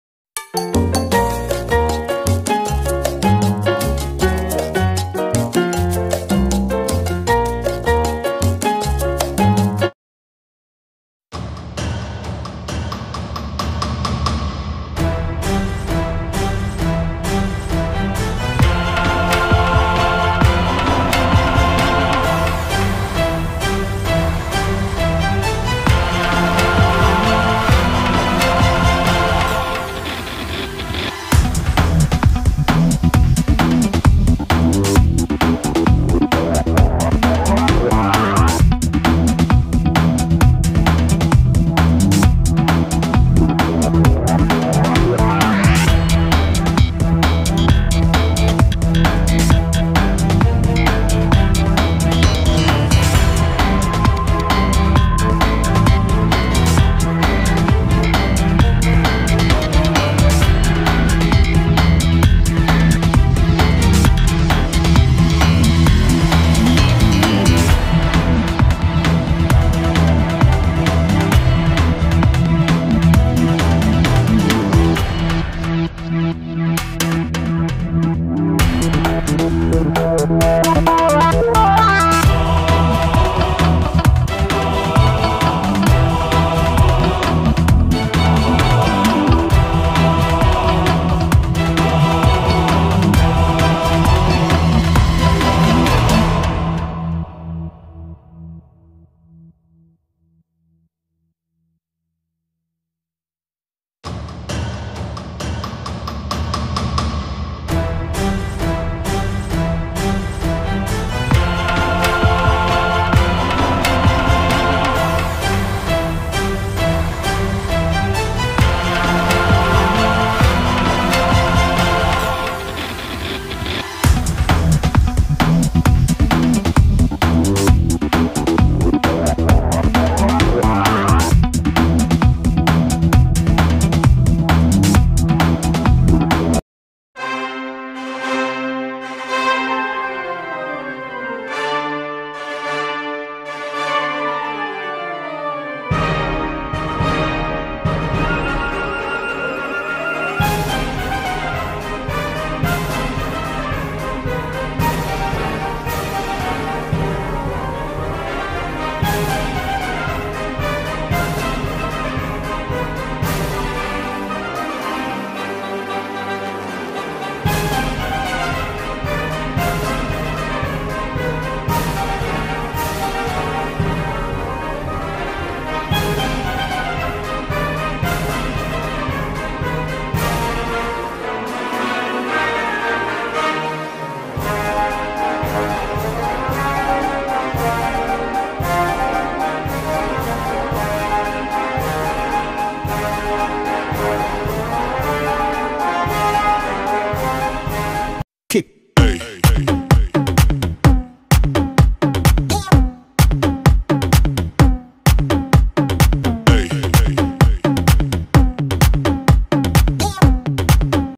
FREEFALL AT DUBAI GLOBAL VILLAGE TALLEST DROP TOWER | DUBAI GLOBAL BURJ | ATE LORN'S CHANNEL
freefall thrilling ride  experience of my daughter at Dubai Global Village